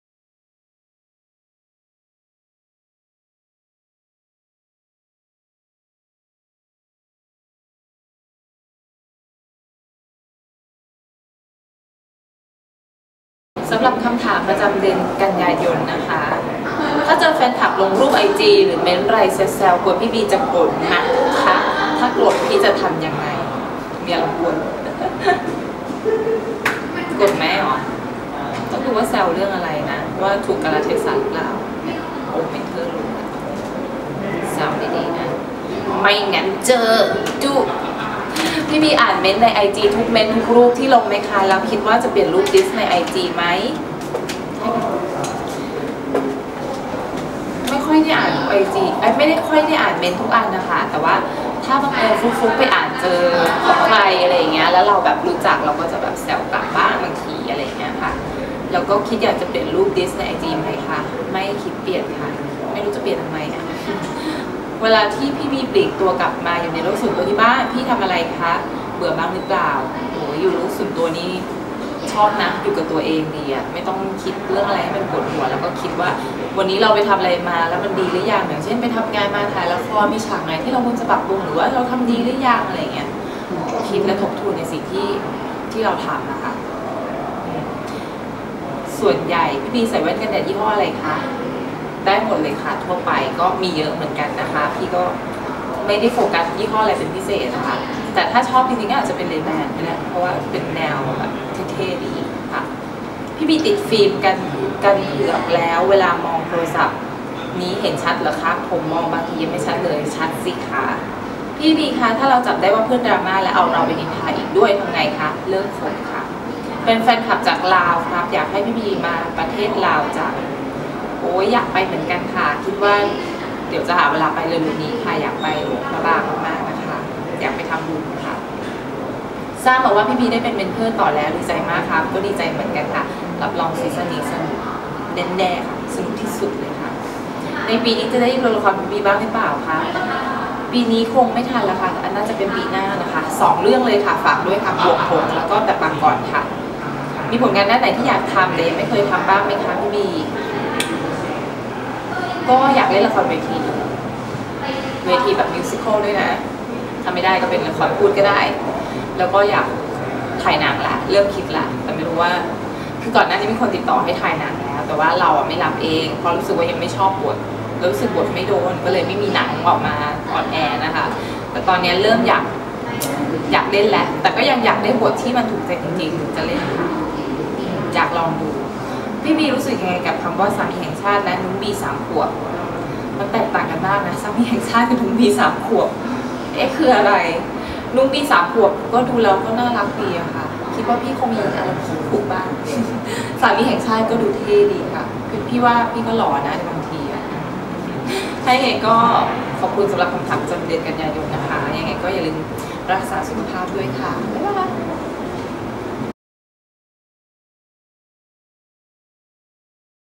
Bee Namthip Clip Update September 2016
บี น้ำทิพย์ ตอบคำถามแฟนคลับ เดือนกันยายน 2559 Bee Namthip official website update September 2016